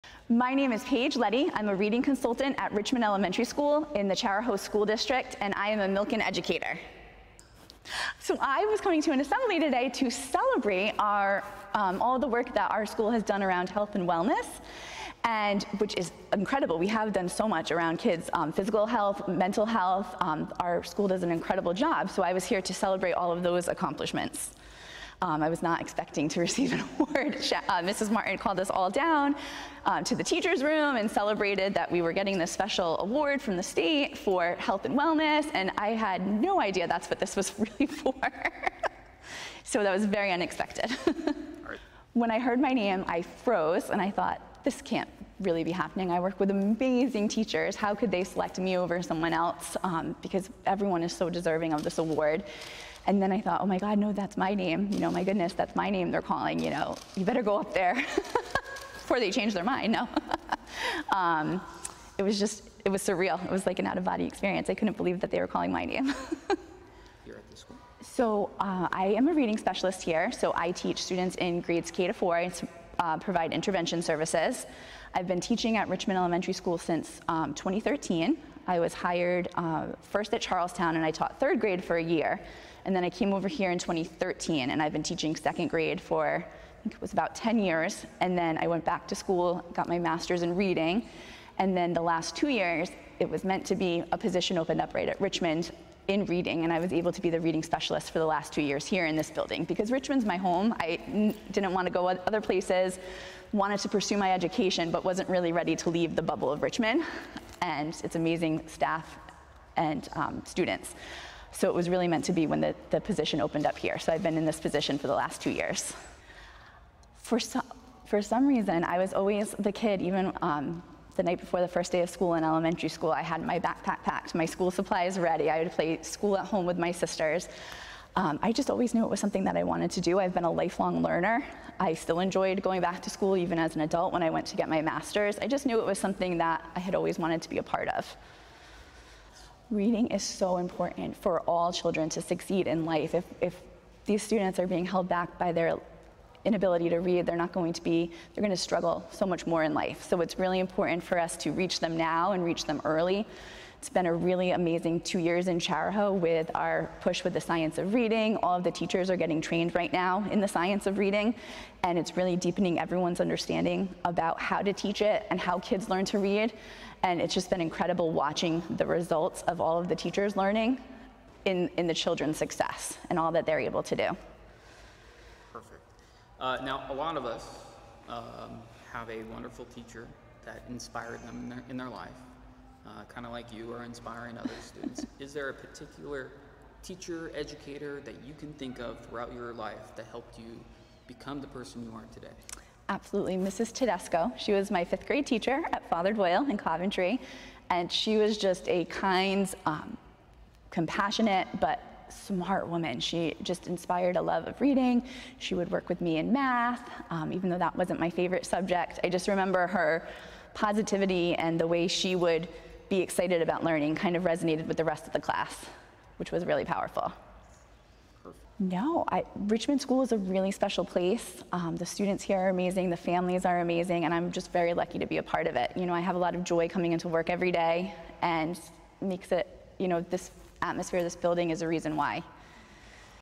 Paige Leddy Milken Award Interview (Richmond RI)
Paige Leddy, reading specialist at Richmond Elementary School, talks with the Milken Family Foundation at her surprise Milken Educator Award notification in Richmond, RI.